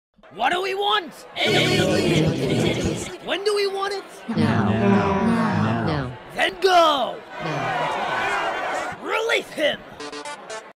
HowArea51Should'veGoneDown
so i made this in a couple of days, im not the proudest of the animation but i do like how i did the audio